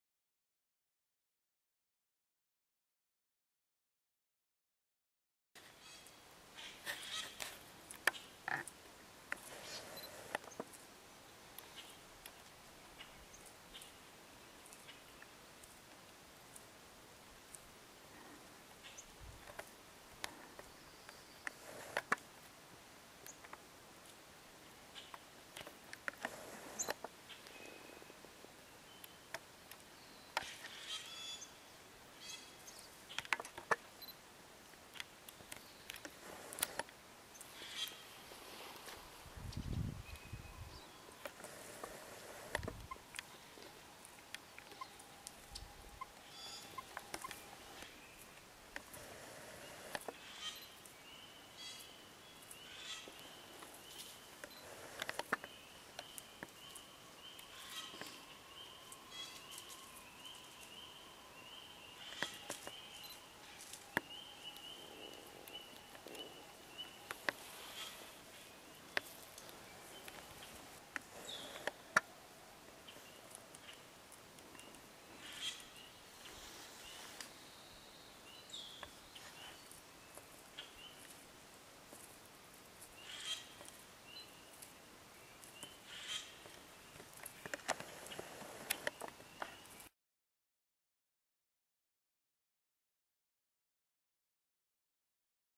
Spruce Grouse
This Spruce grouse was on the trail and when we aproached it flew up into a spruce tree and allowed us to watch it feed.